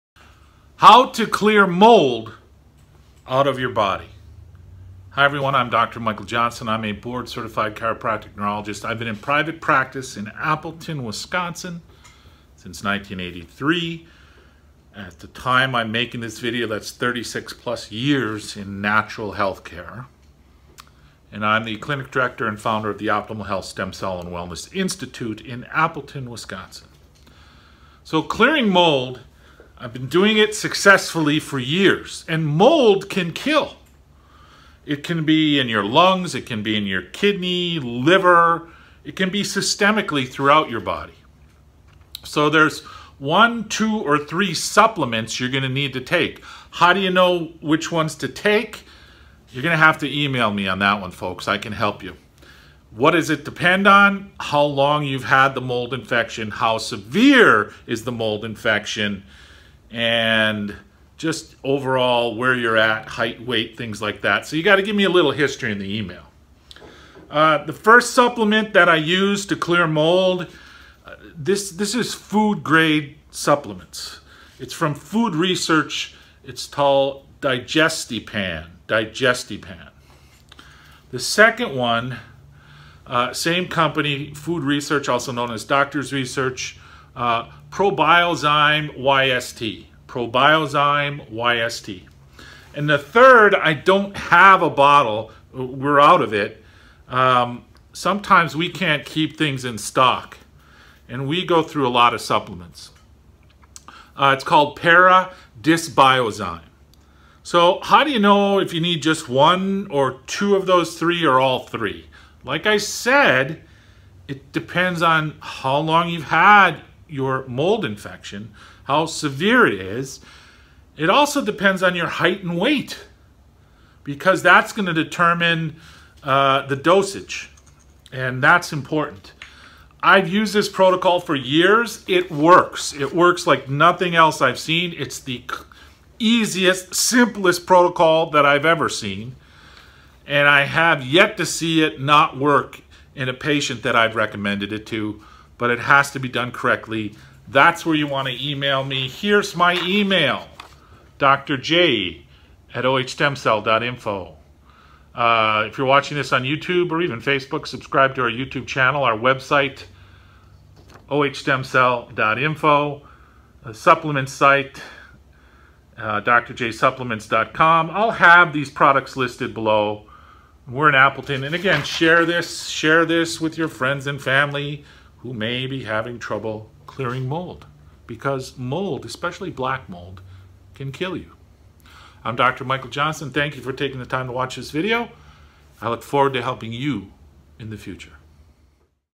MOLD: HOW TO CLEAR MOLD OUT OF YOUR BODY!
It is MORE than just stem cell therapy!   It has to be a wholistic approach which includes complete blood work, hormone testing, trigger testing for toxins and infections, genetic testing and umbilical stem cells injected into the brown fat because it has the highest concentration of mitochondria!

The Optimal Health Stem Cell and Wellness Institute
2000 S. Memorial Dr, Ste 201